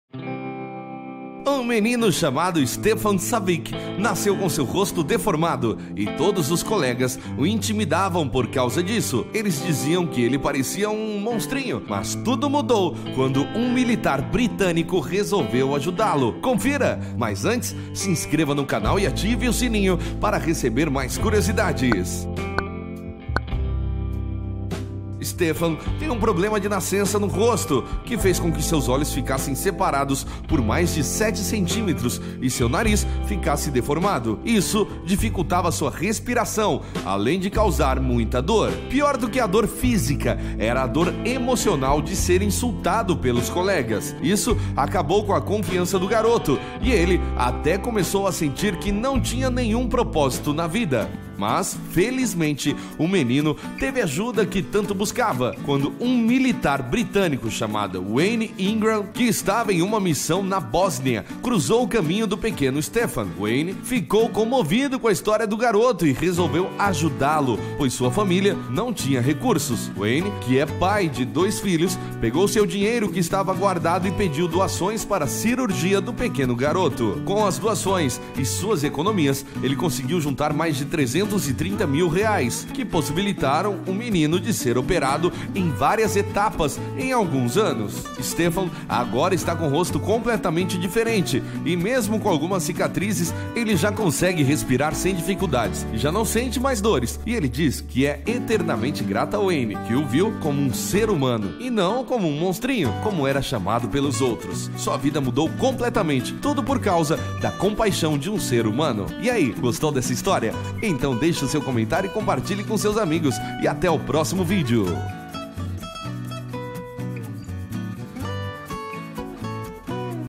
Ele era chamado de "MONSTRINHO", até que um militar mudou sua vida!
Um menino chamado Stefan Savic nasceu com seu rosto deformado, e todos os colegas o intimidavam por causa disso. Eles diziam que ele parecia um “monstrinho”. Mas tudo mudou quando um militar britânico resolveu ajuda-lo confira!